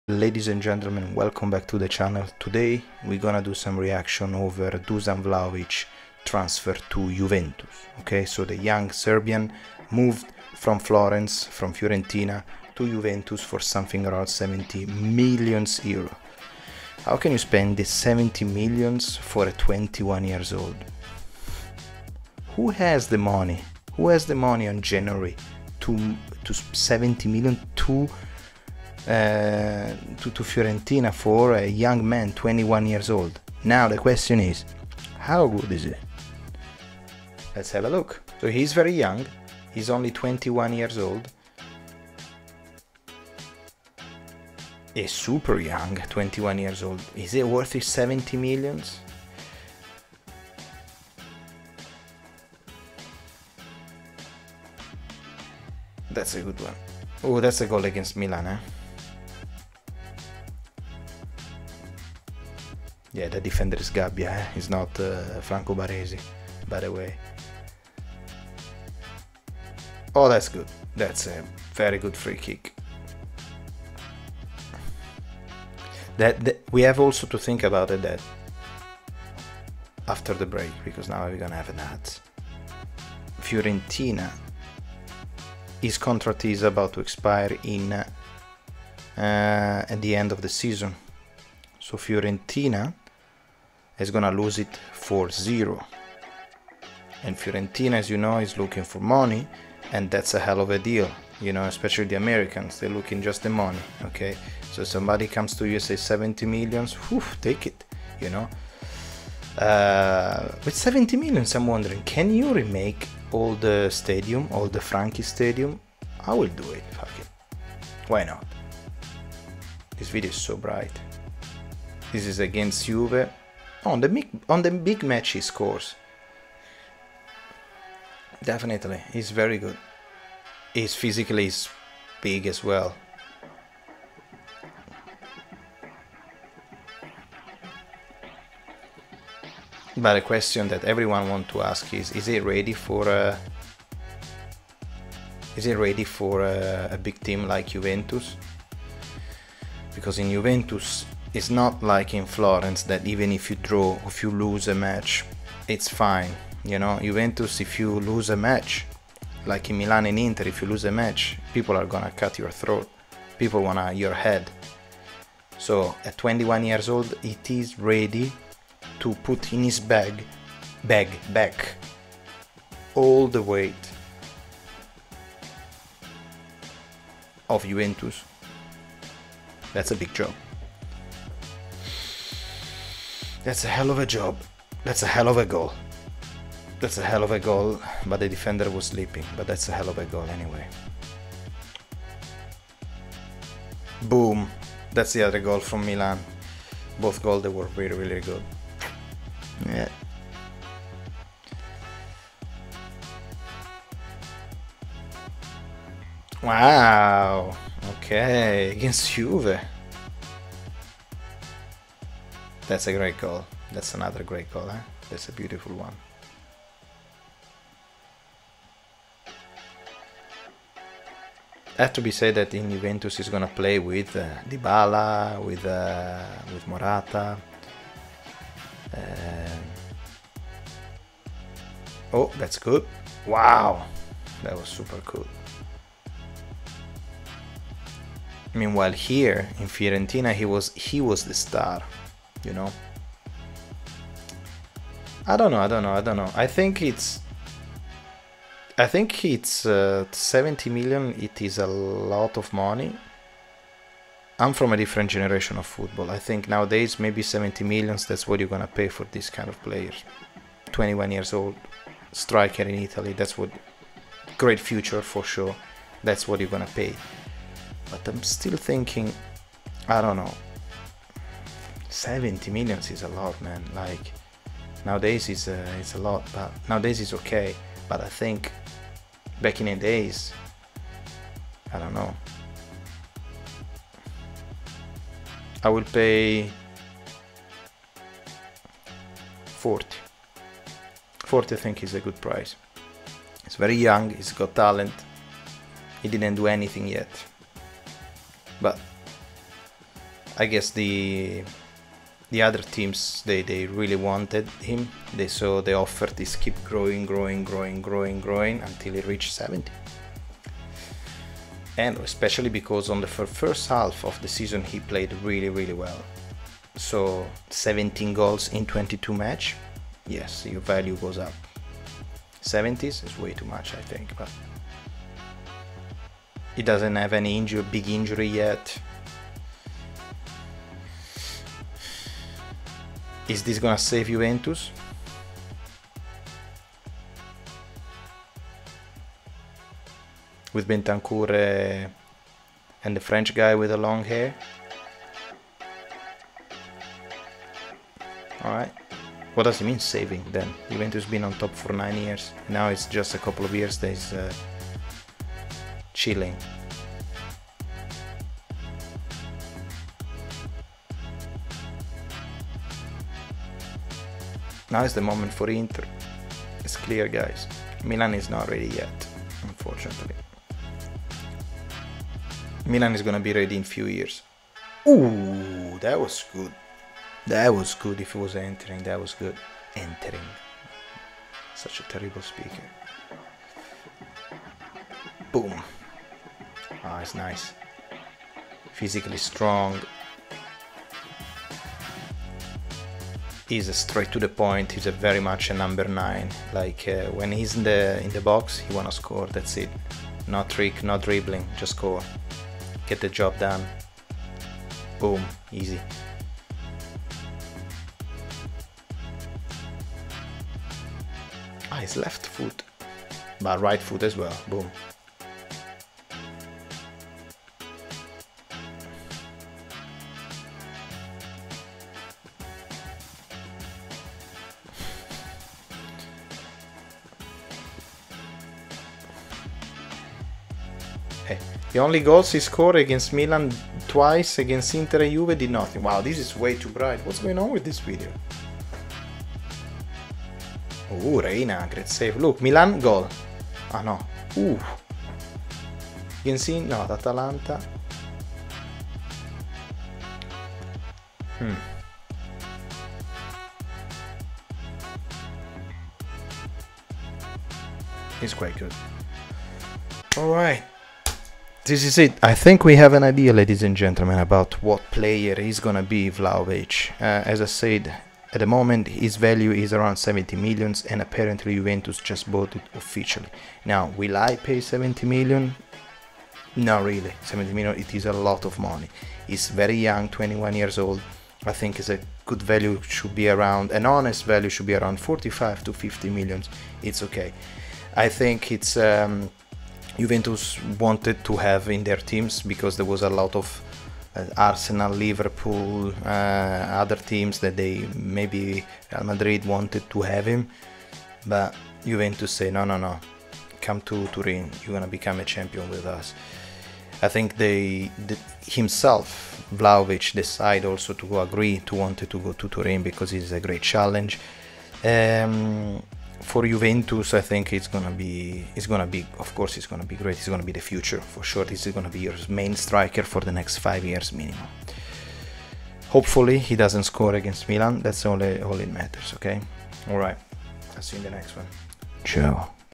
Reaction to new Juventus Player Vlahović, Ac Milan fan
Hey, here I got for you another video reaction to the new Juventus player Vlahovic !
Is a very young striker, full of talent, but is he really worth it 70 M EURO ?
Her efor you some RAW reaction to Vlahovic highlights ! Enjoy